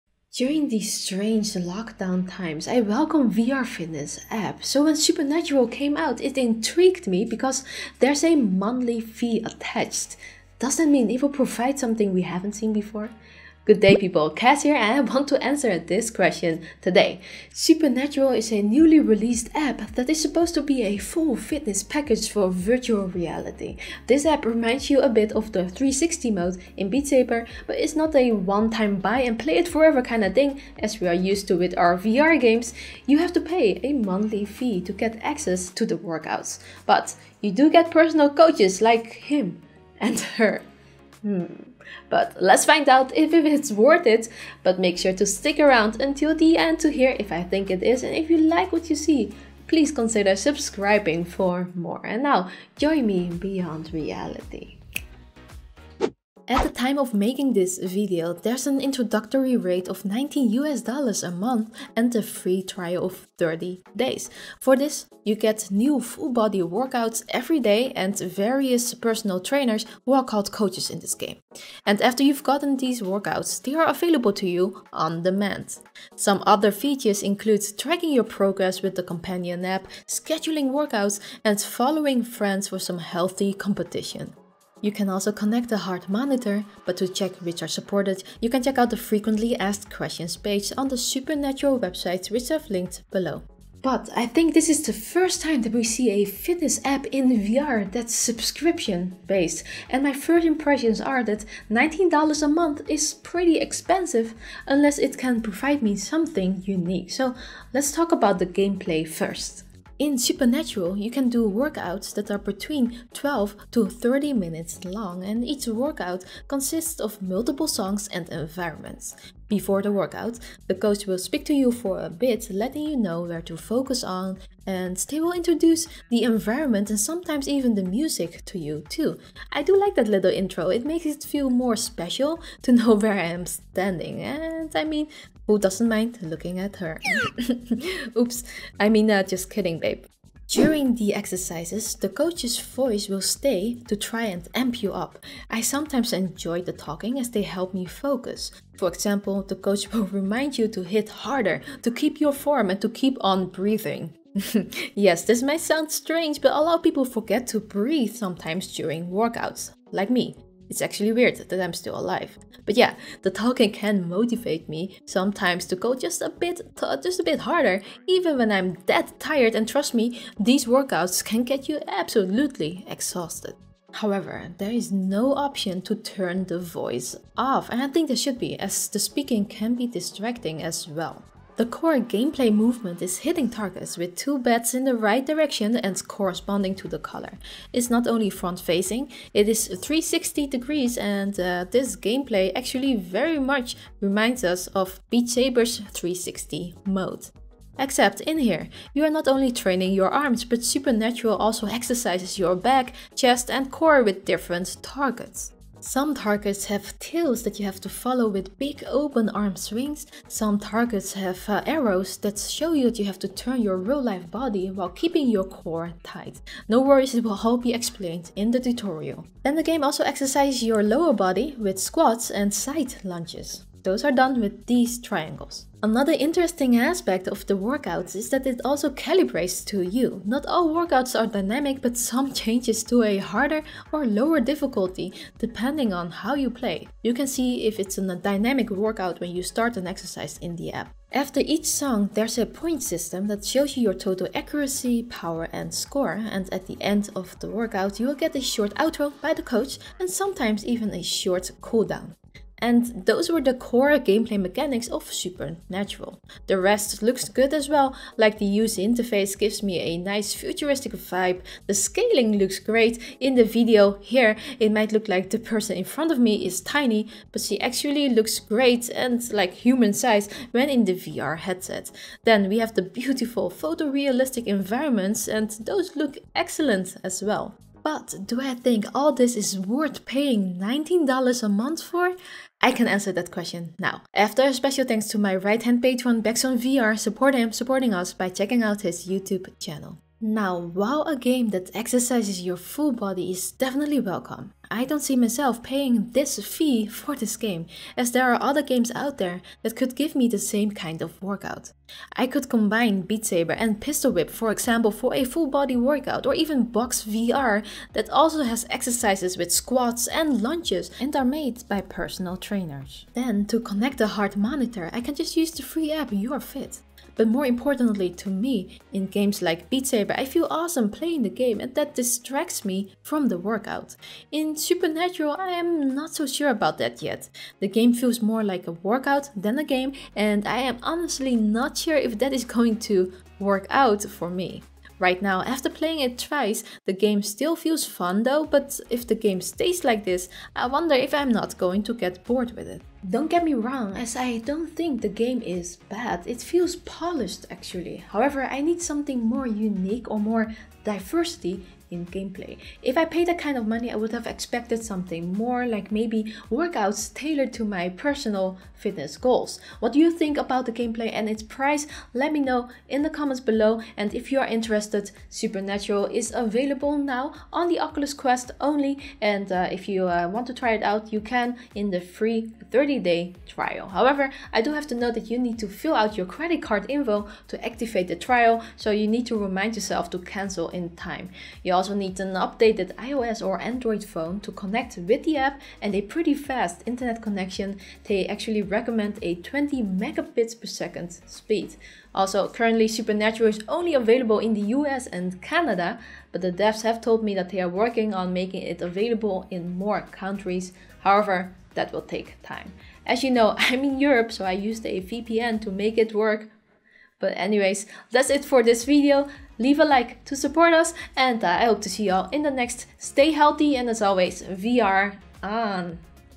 Supernatural VR - Subscription-Based Workout On Oculus Quest... Worth it?
Supernatural is a new subscription-based app that is supposed to be a full fitness package for VR. Let's check if it worth its price!

During these lockdown times, I welcome VR fitness apps. So when Supernatural came out, it intrigued me because there’s a monthly fee attached. Now I want to know if that means if it will provide something we haven’t seen before? In this video, I answer this question!

TIMESTAMPS
00:56 - Supernatural VR Introduction
01:47 - Supernatural VR Core gameplay mechanics
04:50 - Supernatural VR Conclusion

(Coolblue)

OUR GEAR
Our VR equipment

- Wintceas
- Studioform VR
- Andy
- Albert
- Ben P.
- Steve Dunlap

VR on!
- Cas and Chary VR